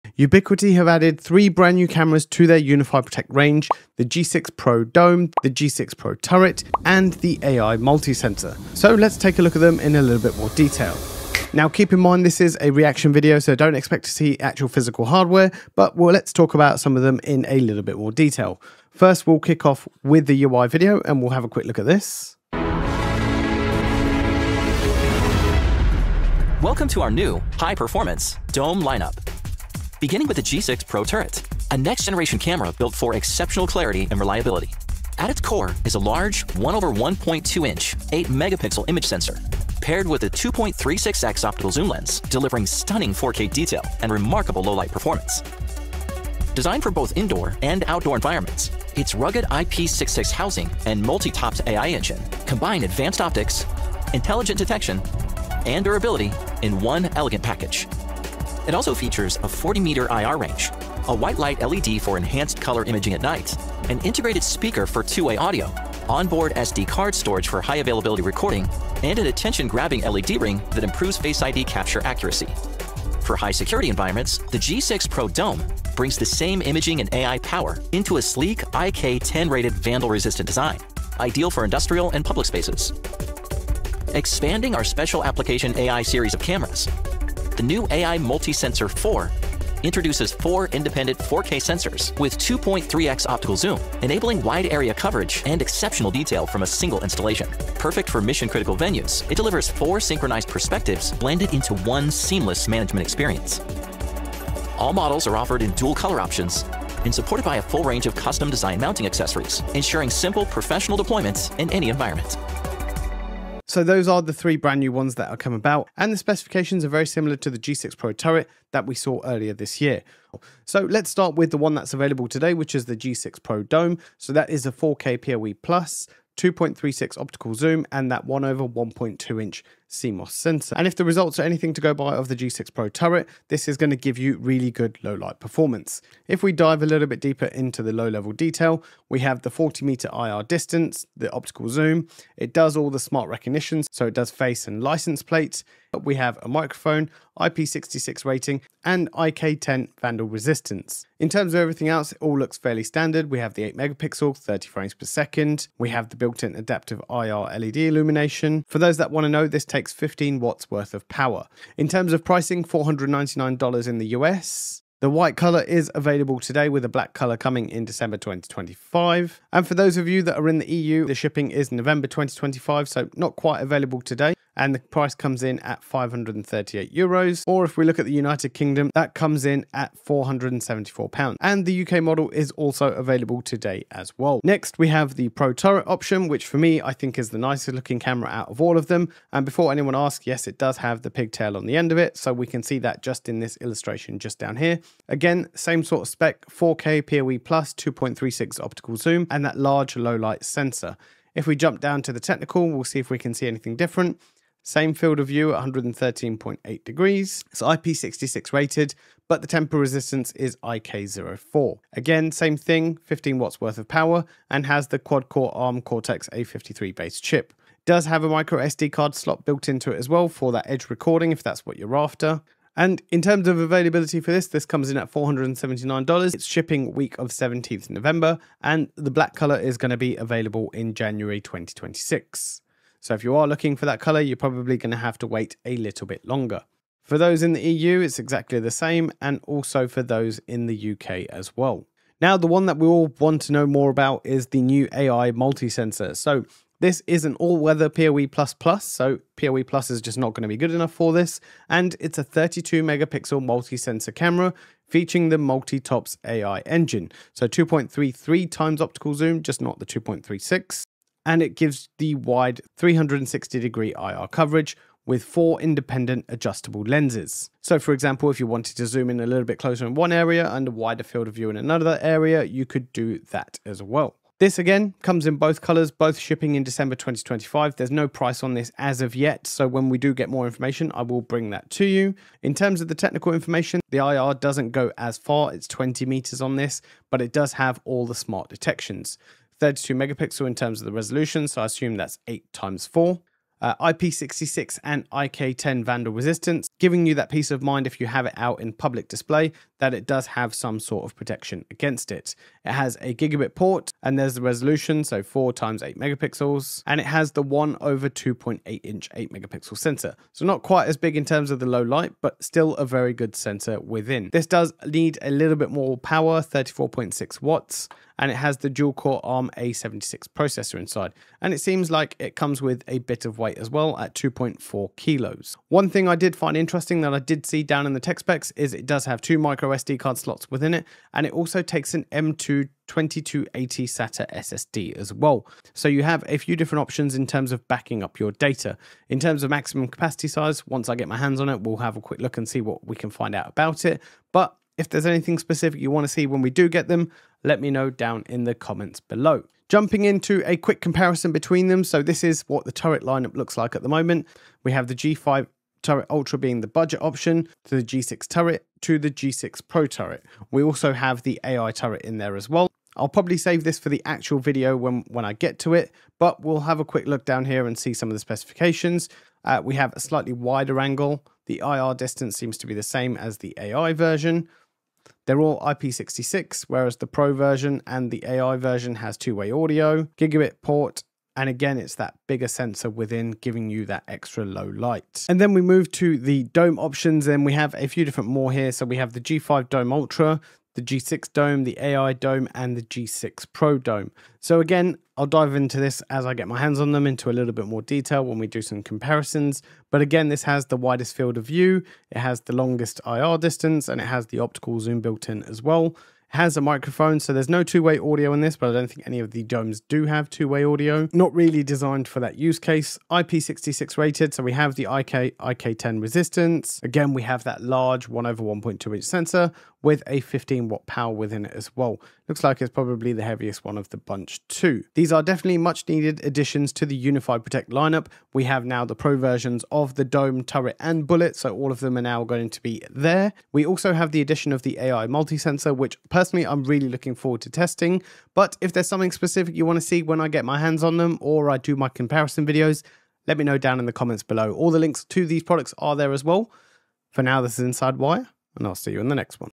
NEW UniFi Protect Cameras | G6 Pro Dome, G6 Pro Turret & AI MultiSensor 4
Ubiquiti just announced their next-generation high-performance cameras — and they look incredible! In this video, I’m reacting to the brand-new G6 Pro Turret, G6 Pro Dome, and AI MultiSensor-4.

From 8MP 4K image sensors and 2.36x optical zoom to advanced AI detection, vandal-resistant design, and multi-sensor coverage — Ubiquiti is taking UniFi Protect to the next level.

We’ll talk about what’s new, how these compare to the G5 and AI lineup, and what this means for installers, prosumers, and anyone deep in the UniFi ecosystem.